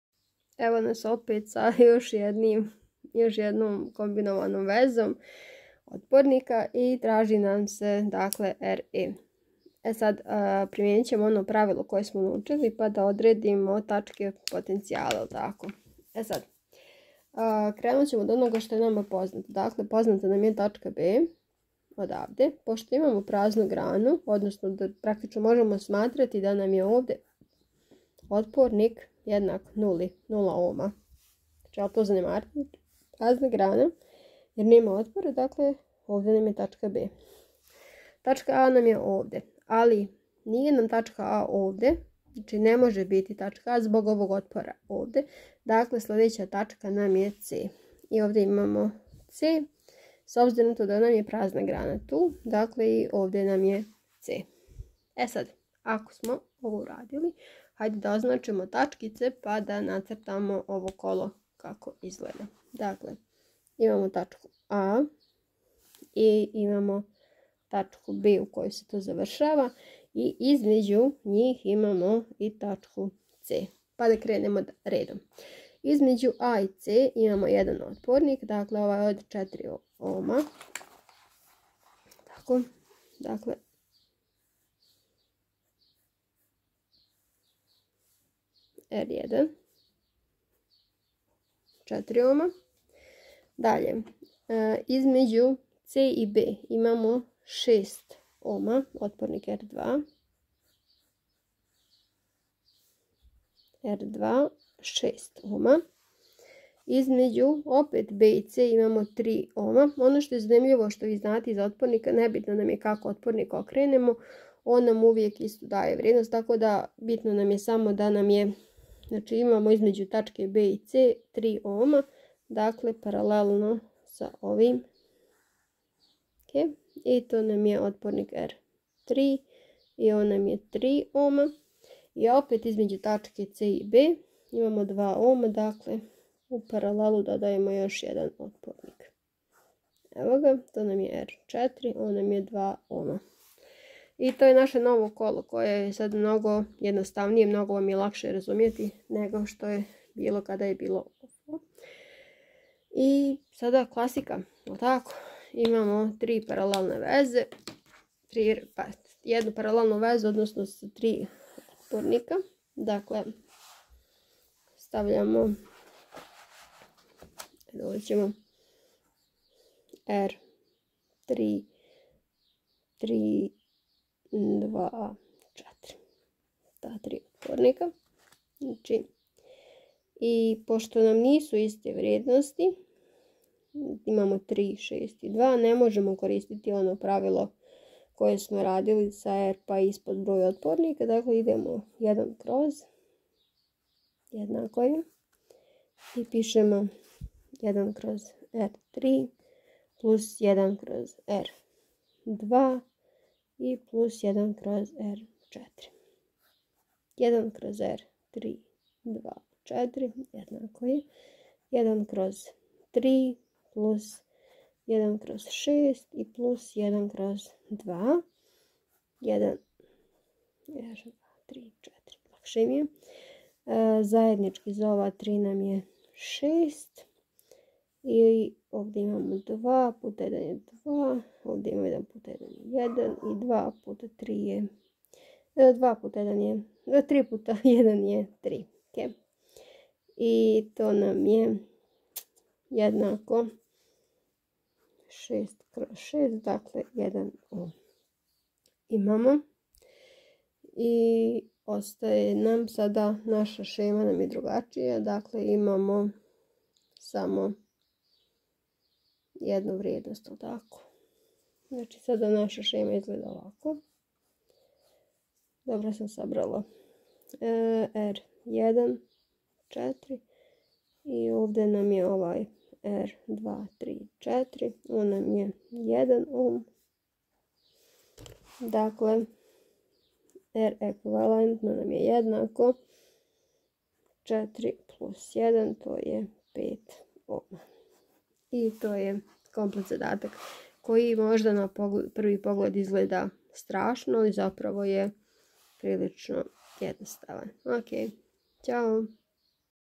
Mini serija:kako izračunati ukupnu otpornost? Primjer br.4
U ovom primjeru takođe imamo četiri otpornika kao u trećem primjeru ali sa razlikom u vezibanju. Nadam se da vam je korisno. :)